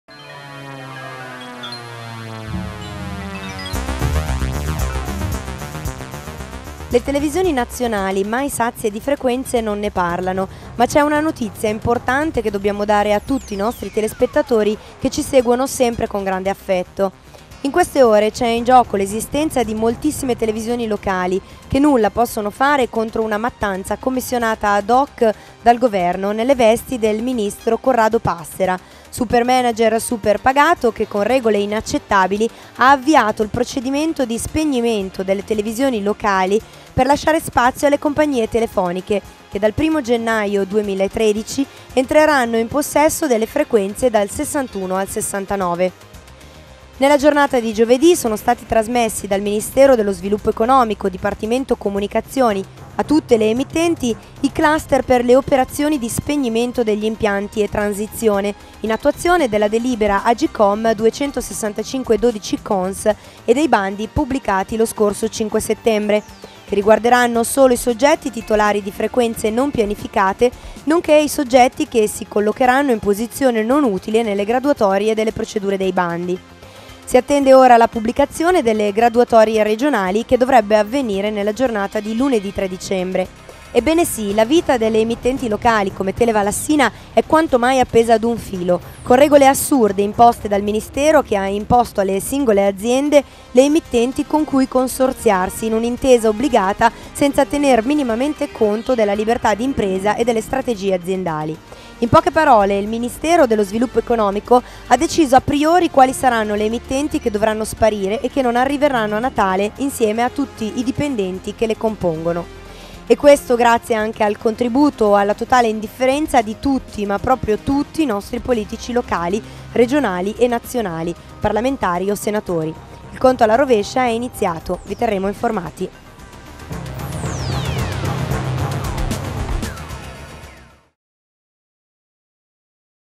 TV LOCALI - Monti e Passera ne hanno decretato la morte
Le TV Nazionali ( Rai, Mediaset, La7 ) non ne parlano... ma c'e' una notizia importante che dobbiamo dare ai nostri telespettatori. In queste ore c'e' in gioco l'esistenza di molte piccole televisioni locali che nulla possono fare contro una mattanza commissionana ad hoc dal Governo Monti nelle vesti del ministro Corrado Passera ( supermanager superpagato ) che con regole inaccettabili ha avviato il procedimento di spegnimento delle televisioni locali per lasciare spazio alle compagnie telefoniche che dal 1° gennaio 2013 entreranno in possesso delle frequenze dal 61 al 69.
La vita delle emittenti locali come Televallassina e' quantomai appesa ad un filo con regole assurde imposte dal Ministero che ha imposto alle singole aziende le emittenti con cui consorziarsi in un'intesa obbligata senza tener conto minimamente della liberta' d'impresa e delle strategie aziendali. In poche parole, il Ministero dello Sviluppo Economico ha deciso a priori quali saranno le emittenti che dovranno sparire e che non arriveranno a Natale insieme a tutti i dipendenti che le compongono. E questo grazie anche al contributo o indifferenza totale di TUTTI, MA PROPRIO TUTTI i nostri politici locali, regionali e nazionali ( parlamentari e senatori ). Vi terremo informati